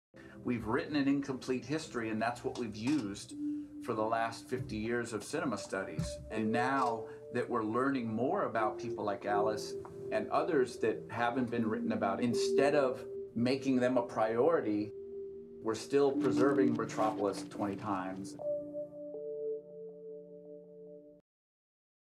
Dino Everett / BE NATURAL: THE UNTOLD STORY OF ALICE GUY-BLACHÉ
Alice Guy-Blaché, the motherofcinema, was there at the beginning and many more followed in her footsteps. It was the norm and it should be now.